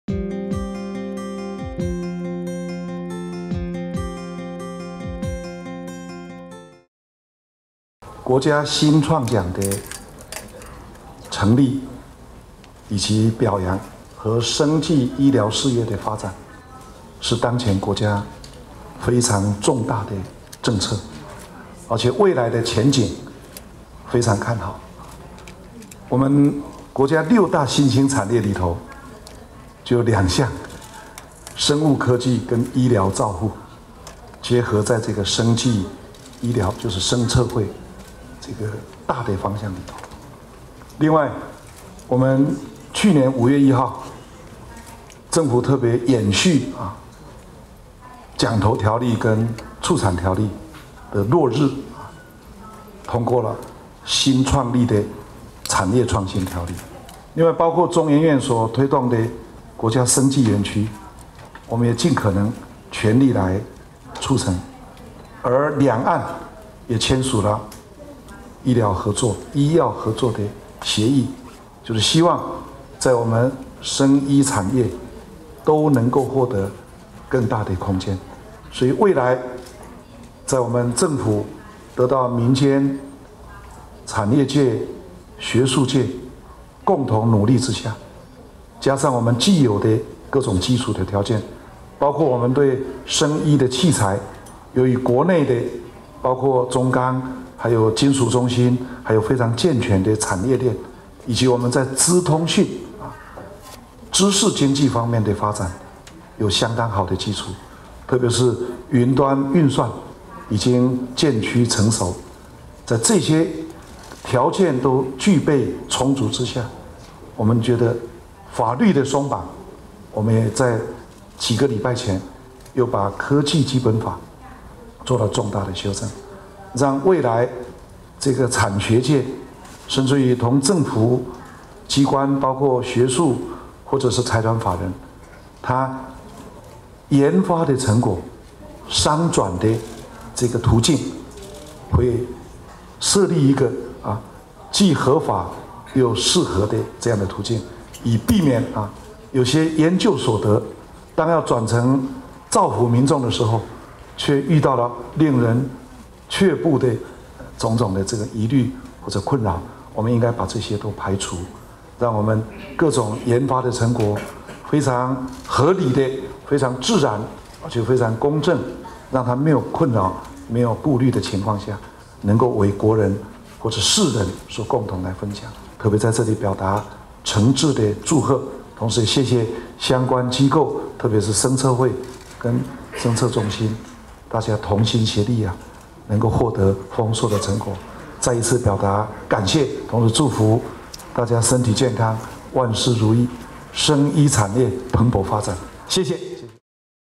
行政院長吳敦義出席第8屆國家新創獎頒獎典禮暨生技研發成果技術論壇
行政院長吳敦義今（4）日出席第8屆國家新創獎頒獎典禮暨生技研發成果技術論壇時表示，新創獎的成立與表揚，以及生技醫療事業的發展，是當前國家重大的政策，未來前景也都看好。他強調，國家六大新興產業中有生物科技及醫療照護兩項，能結合成為生技醫療，這也是國家生技醫療產業策進會發展的方向。

吳院長指出，政府去年將營所稅從25%調降到17%，凡是有創新，都可獲抵減或獎勵，也就是國家在法律上獎勵創新，而六大新興產業當中，就有生物科技及醫療照護。此外，中研院推動的國家生技園區，政府也會全力促成，而兩岸也簽署了醫藥合作的協議，因此生醫產業未來有很大的發展空間，例如新竹竹北設立的生醫中心，因具有健全的產業鏈，以及資通訊的日趨成熟，已讓生技及醫療照護奠定了良好基礎。

吳院長說，政府近期將科技基本法做了重大的修正，讓未來產學界，甚至政府機關，包括學術或財團法人所研發的成果，都會設立合法又適合的途徑，使研發成果能合理及在沒有顧慮的情況下，讓國人或世人分享，例如今天表揚的36項新創獎的項目，就有令人非常驚艷的成果，包括人工牙根、原創藥與植物新藥等。

國家生技醫療產業策進會表示，今年自150多項技術中，評選出企業組5項、學術研究組25項及學生組6名獲獎技術，領域涵蓋新藥、診斷、機電、食品及農業等。